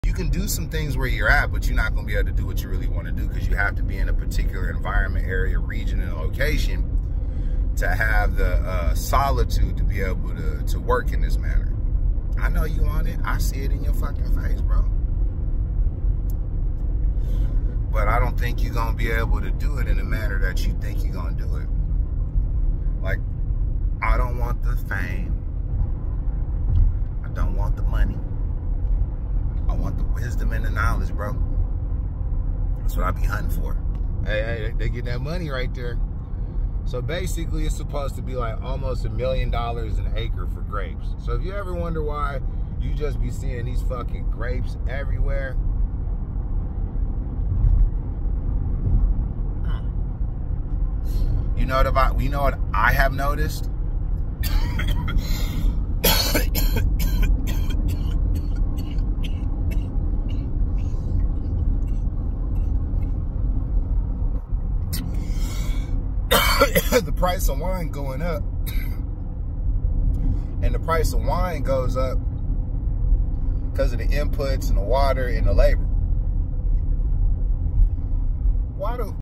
Unlocking Unlimited Potential: Achieving Your Dreams in the Right Environment
Discover the secret to pursuing your passions and finding success in your desired field. This video uncovers the importance of location, solitude, and the pursuit of wisdom and knowledge, rather than fame and wealth. Join us on this inspiring journey of self-discovery.